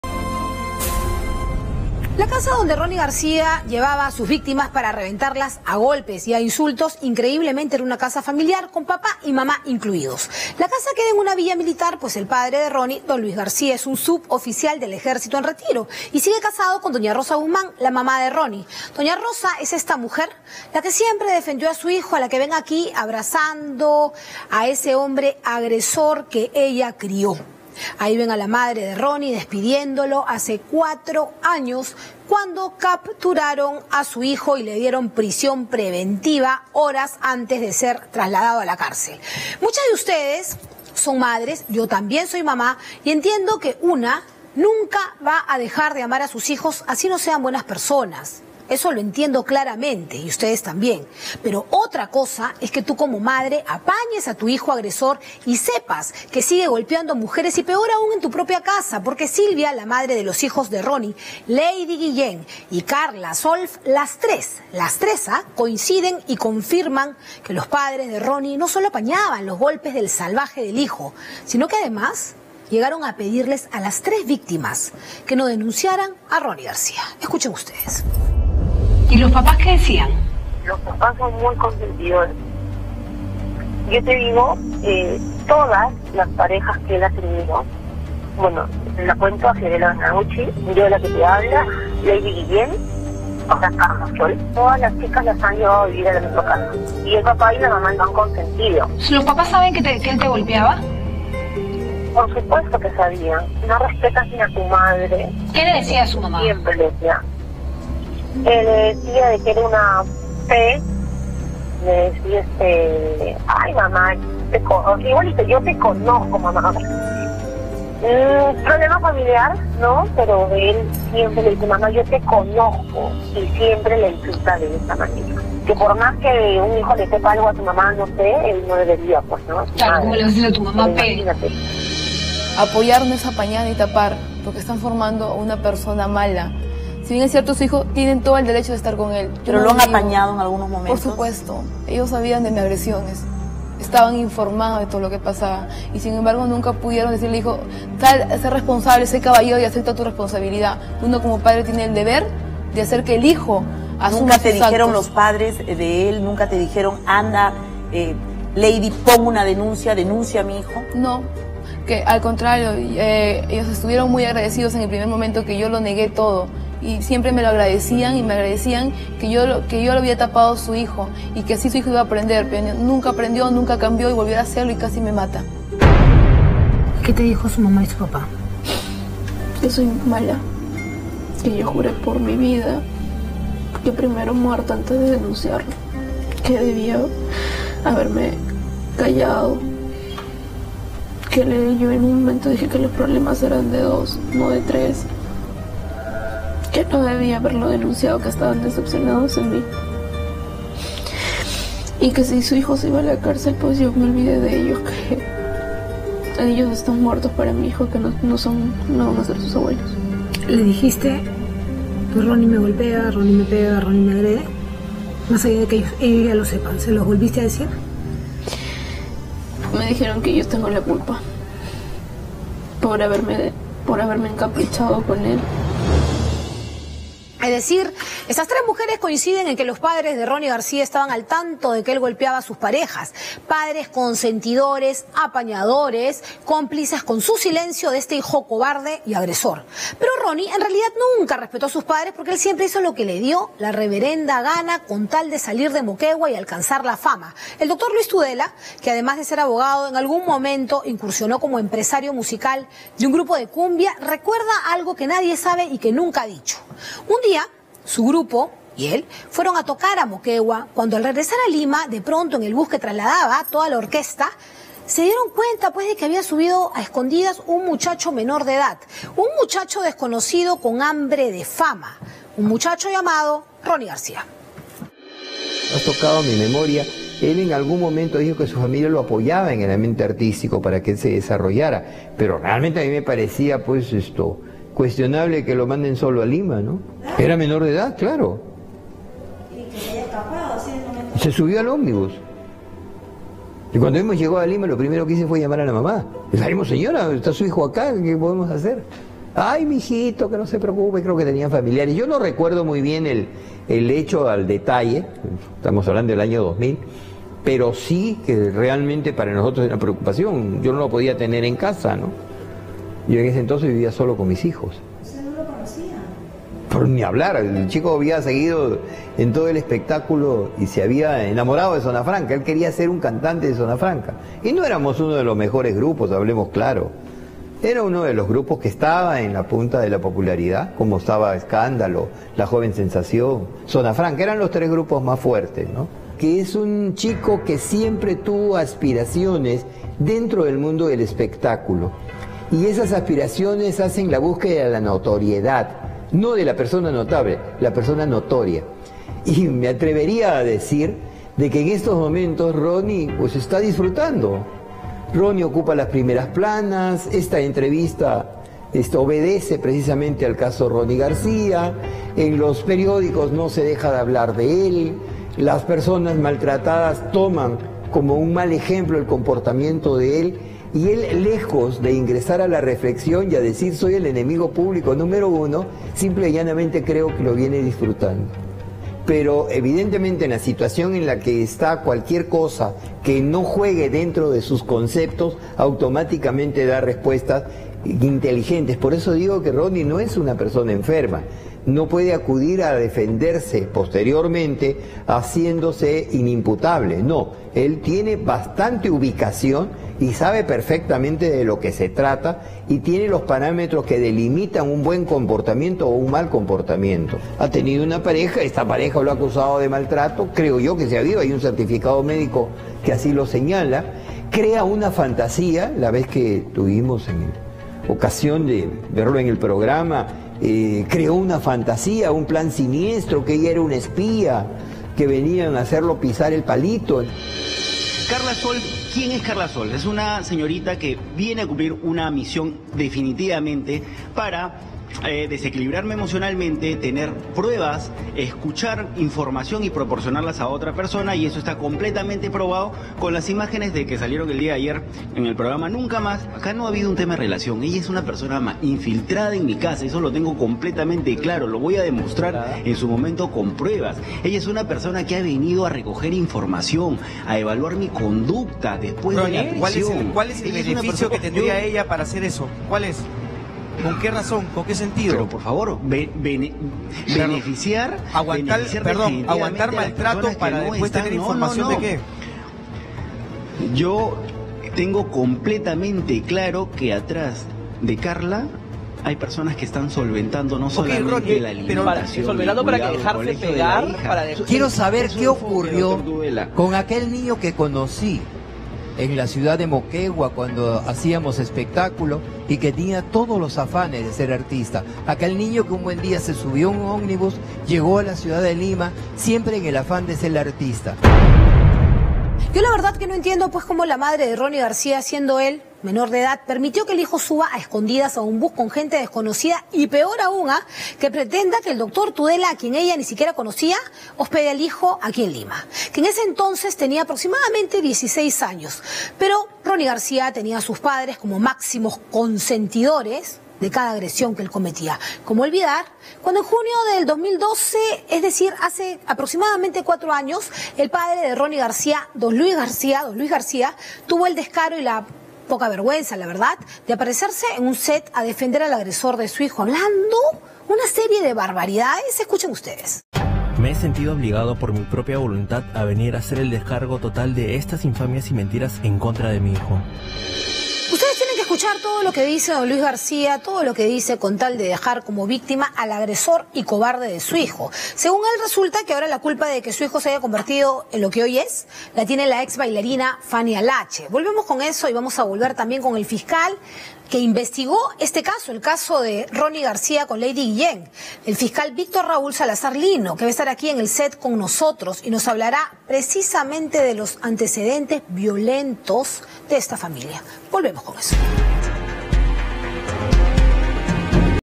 Familia de Ronny García era cómplice de sus agresiones
La casa que utilizaba el músico para insultar, amenazar y golpear a sus víctimas, increíblemente era un hogar familiar, en el que vivía su papá y su mamá. Quiénes se encargaban de ser los cómplices silenciosos de cada una de las agresiones que realizó Ronny García e incluso intimidaban a sus víctimas con amenazas. Recordemos que ellos estaban en conocimientos de estas actitudes de su hijo y jamás hicieron algo para defender a sus parejas, todo lo contrario, intentaban esconder la verdadera cara de Ronny García.